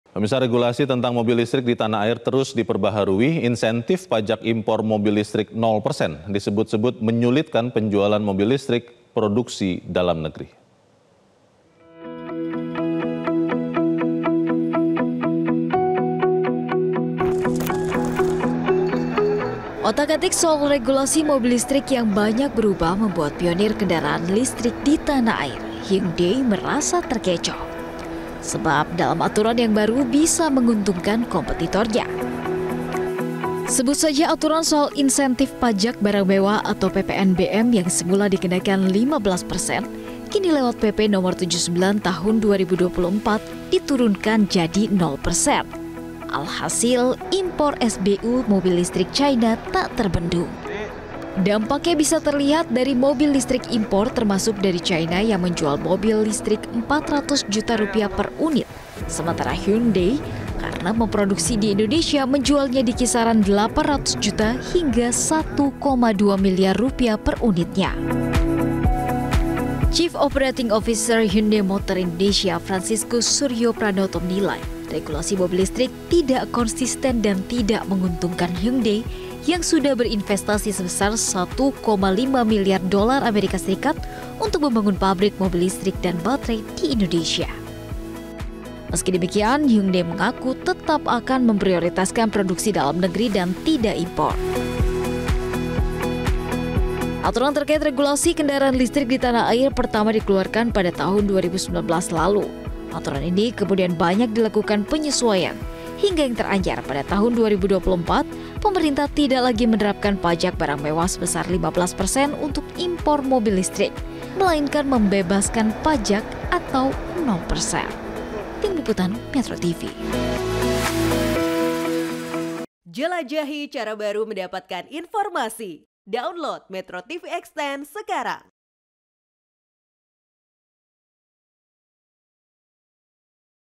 Hyundai Terkecoh Insentif Baru Mobil Listrik
MetroTV, Regulasi Tentang Mobil Listrik Di Tanah Air Terus Diperbarui Insentif Pajak Impor Mobil Listrik Nol Persen Disebut Menyulitkan Penjualan Mobil Listrik Produksi Indonesia